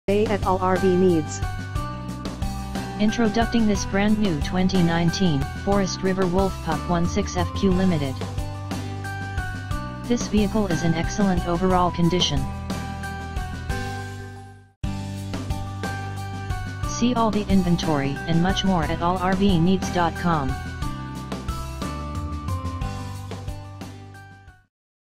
2019 Forest River Wolf Pup 16FQ Limited for sale in MEDFORD, OR
The perfect travel trailer for the weekend get aways, to the one week vacations Forest River has you covered with the 2019 Wolf Pup. The easiest trailer to tow in itsc class. Staying at such a low weight, and tow length you will be amazed at all you get in this 16FQ Wolf Pup. Big quuen bed, large 8.0 cubic foot fridge, booth dinette, full kitchen (stainless steel), and an enormous full bathroom. New to the 2019's: 8.0 Fridge, and more wardrobe storage above, and below the bed. Plus a whole lot more with the limited package ordered.

Or come in today at: 3800 S. Pacific Hwy Medford, Or 97501